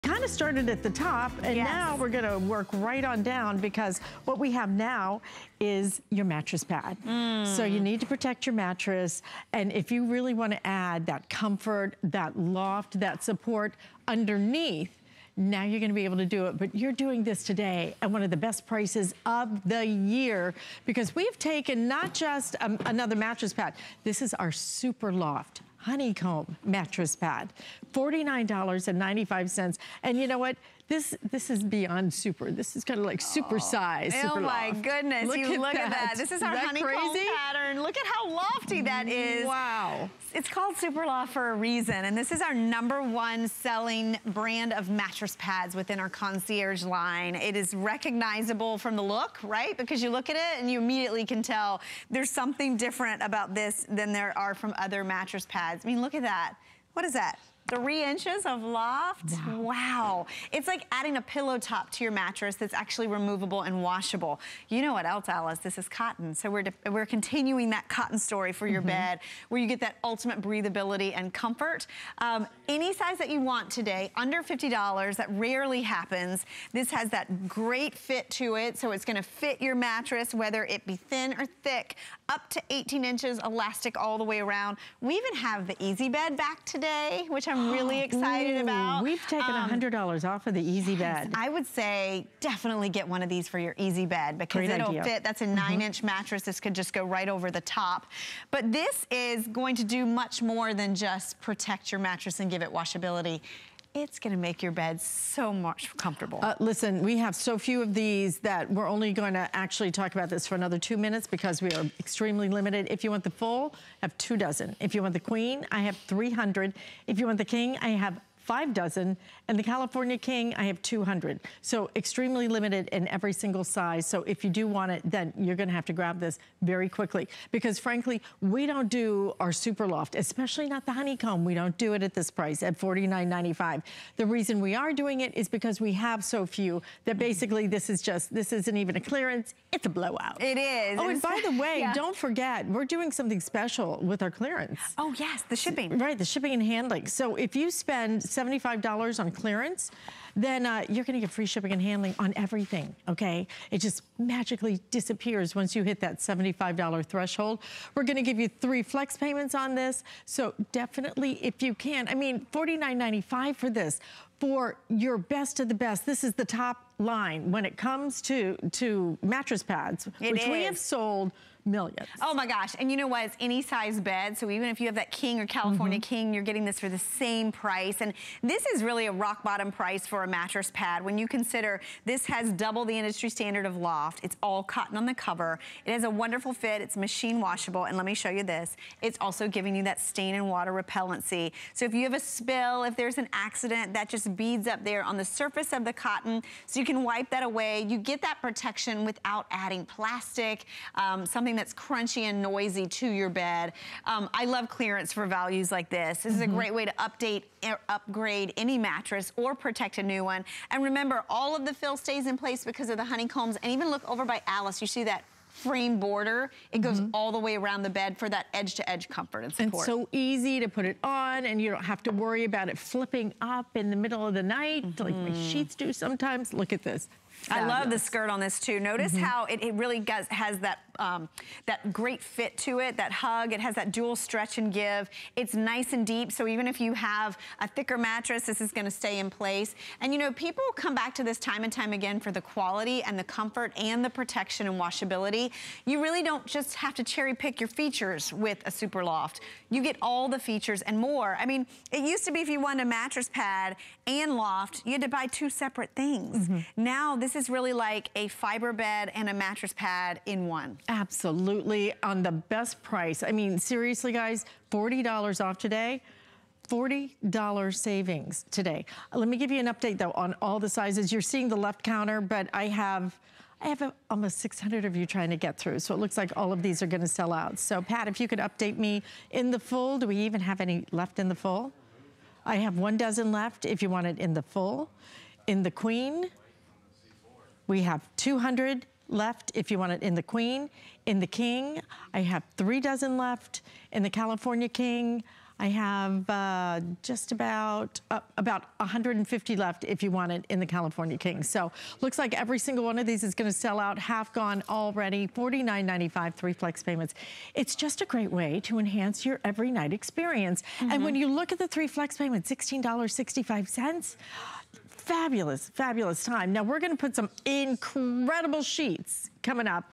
Concierge Collection SuperLoft Honeycomb Mattress Pad
A good night's sleep is sweeter than honey. So it's time you let your bed indulge. This "super pad" cloaks your current mattress with a...